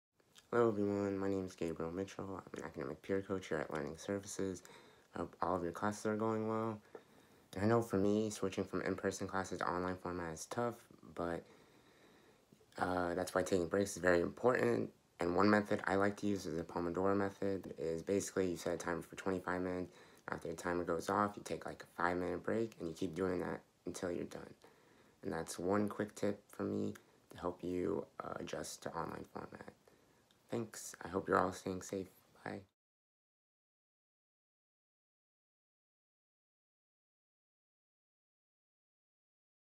Pomodoro Method
The Pomodoro Method, an useful study strategy to help reduce procrastination and keeping on task when you're studying. Gabe teaches us that here, in this quick video!